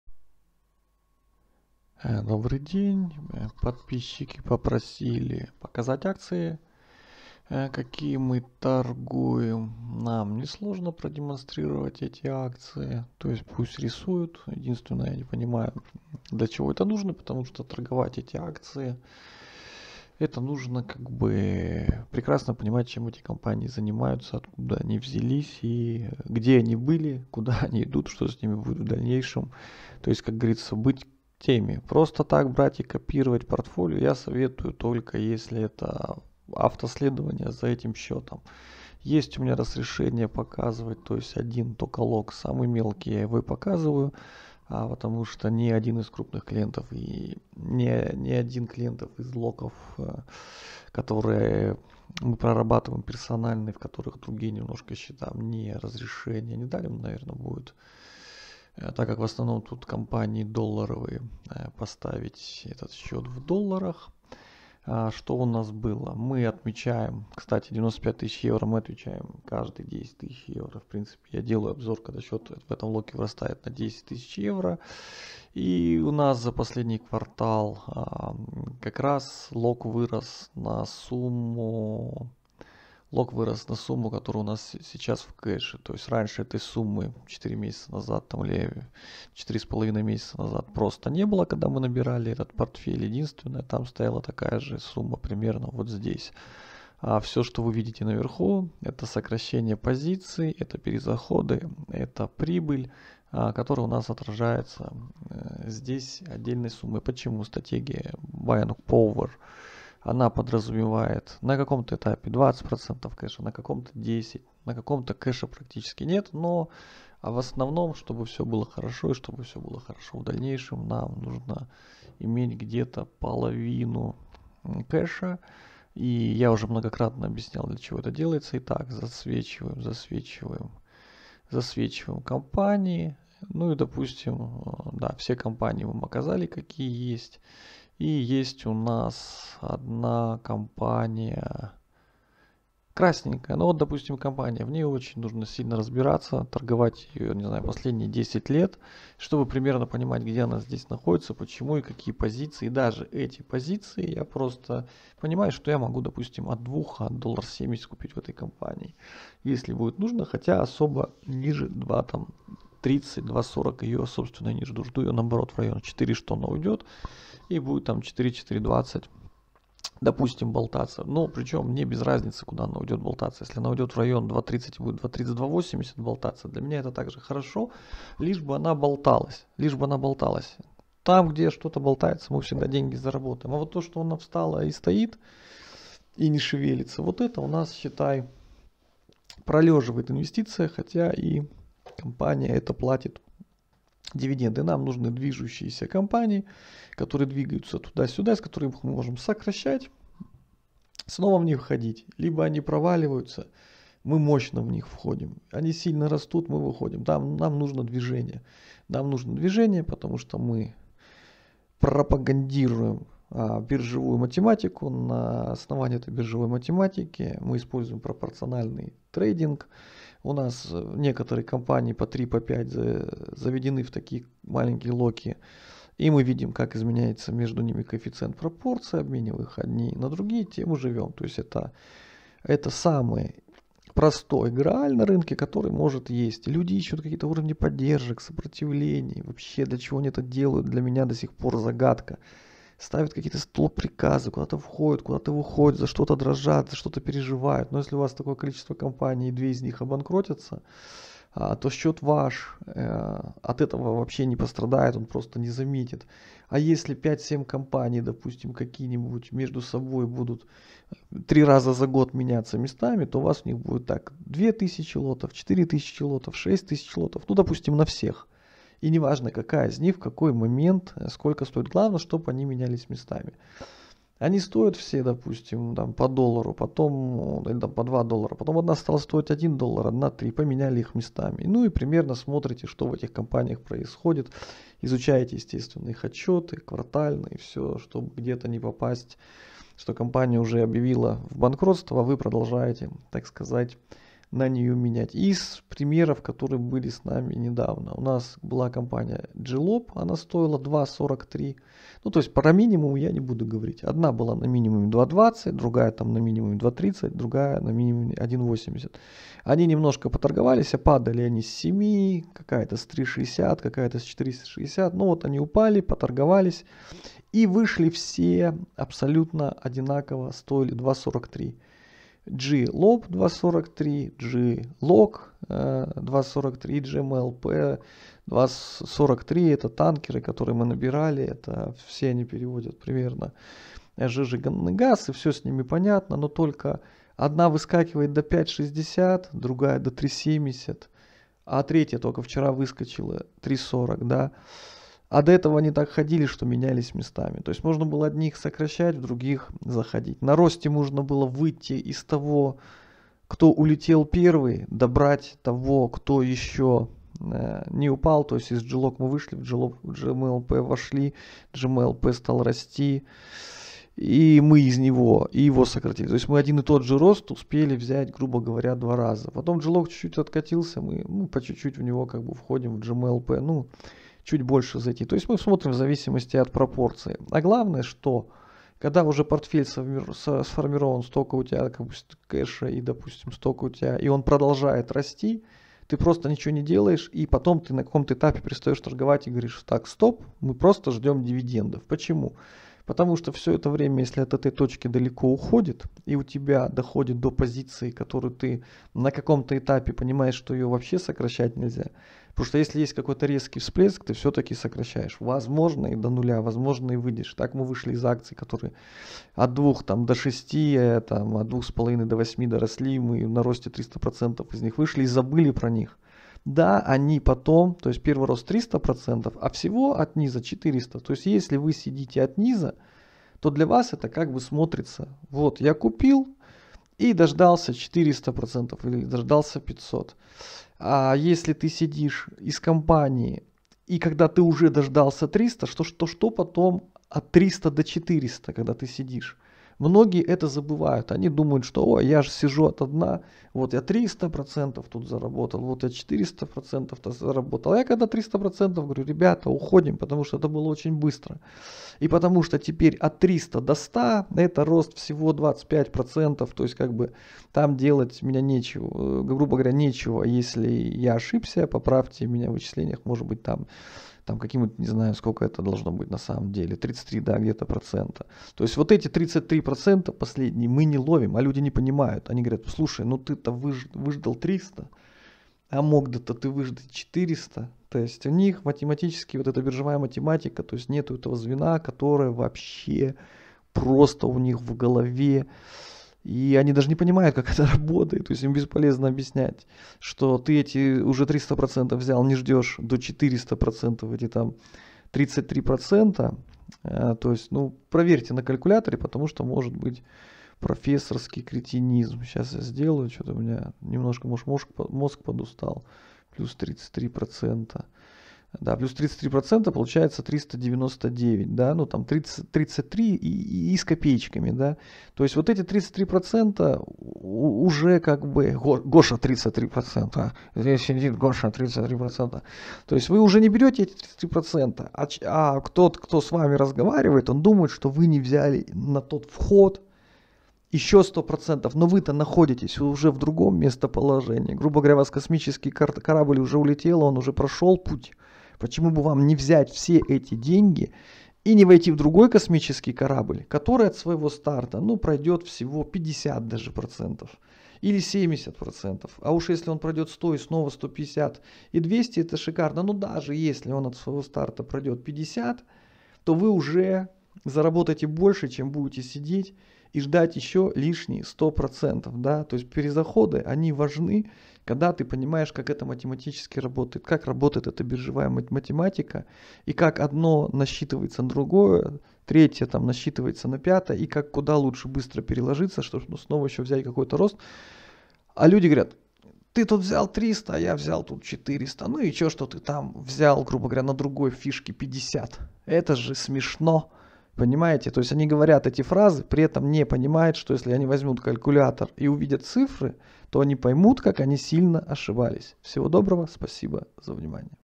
Биржевая математика в действии.
1. Светим один из портфелей.
2. Объясняем, что нужно, чтобы его копировать.
3. Пропорциональный трейдинг.
4. Стратегия "мощность покупки".
5. Как работают проценты на росте.
6. Как правильно перекладываться и почему.